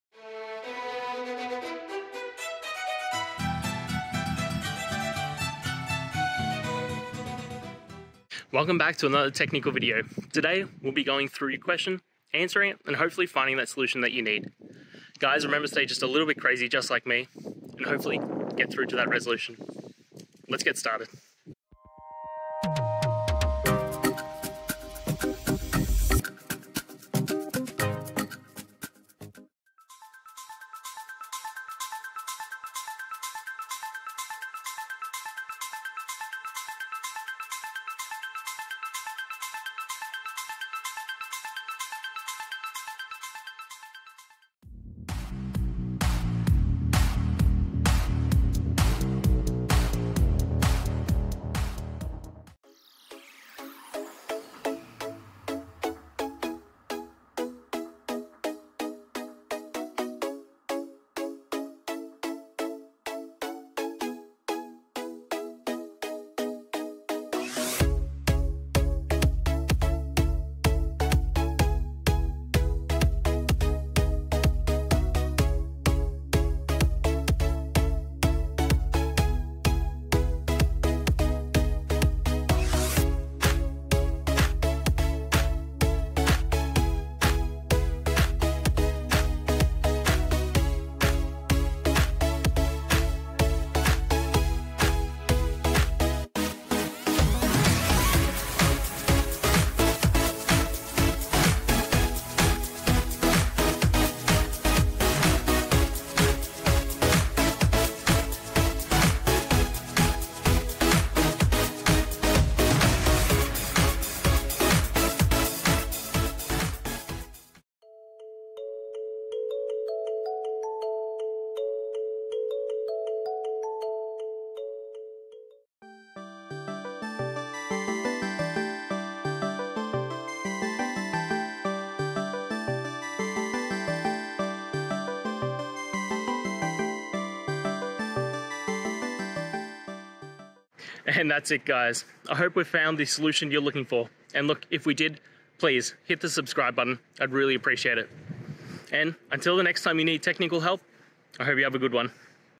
Jmeter Http proxy server throws java.net.URISyntaxException: Illegal character in query at index...
java: Jmeter Http proxy server throws java.net.URISyntaxException: Illegal character in query at index error

Related to: java, jmeter, urlencode, put